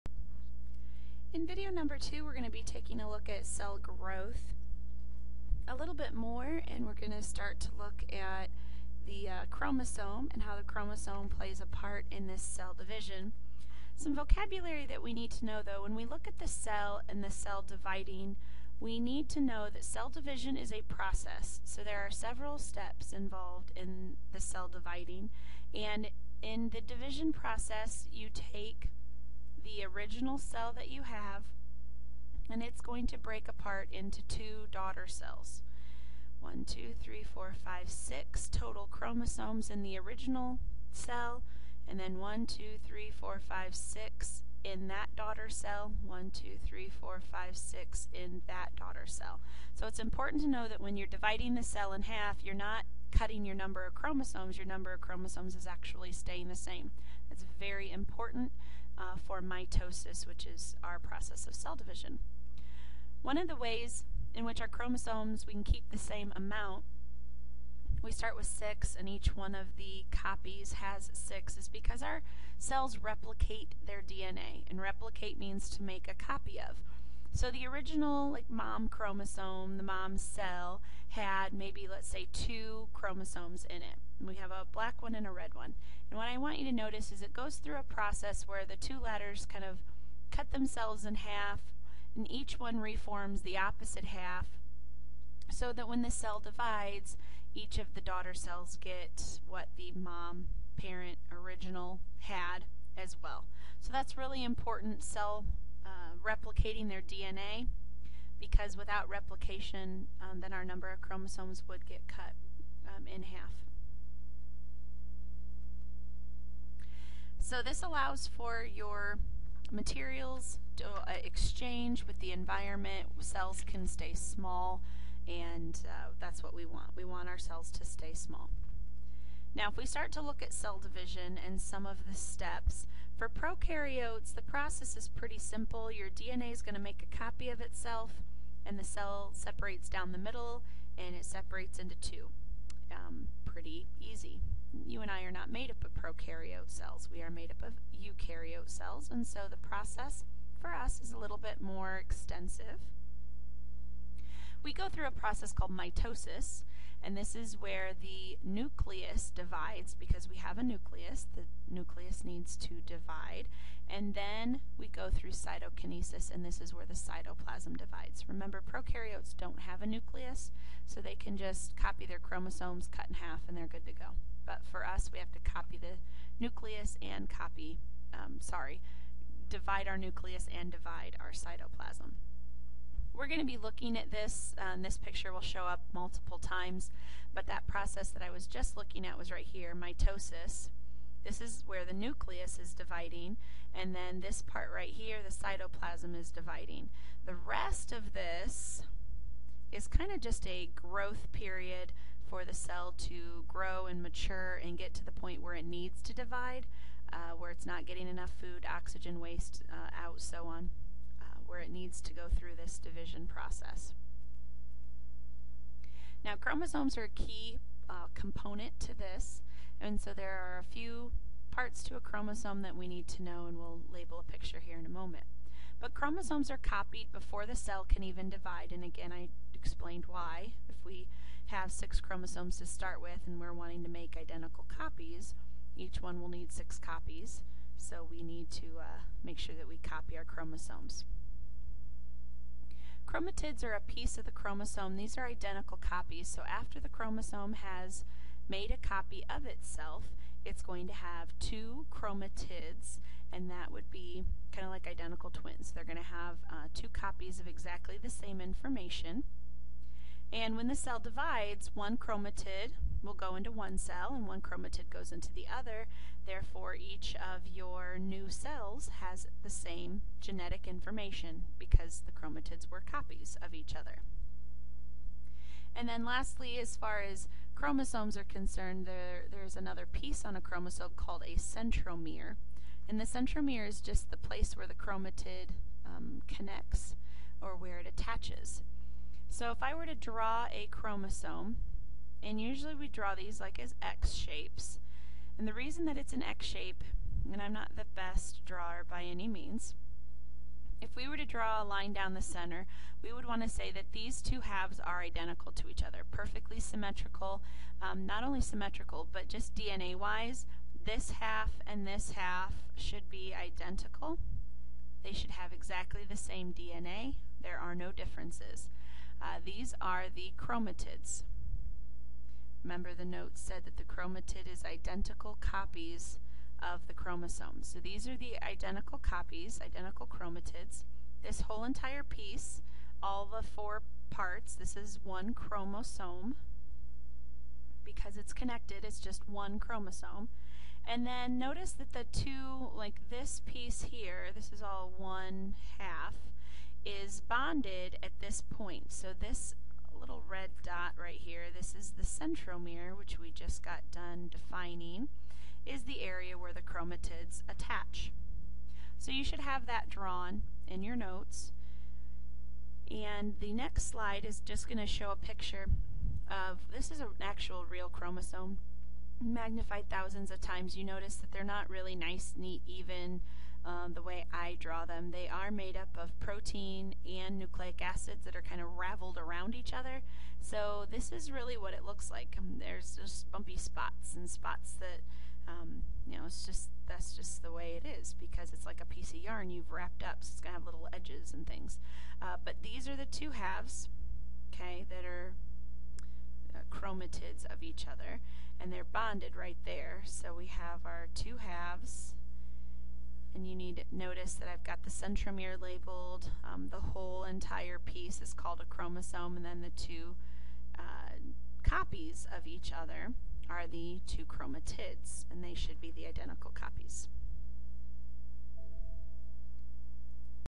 Video 2 Cell Division and Chromosomes
Cell division and labeling parts of a chromosome.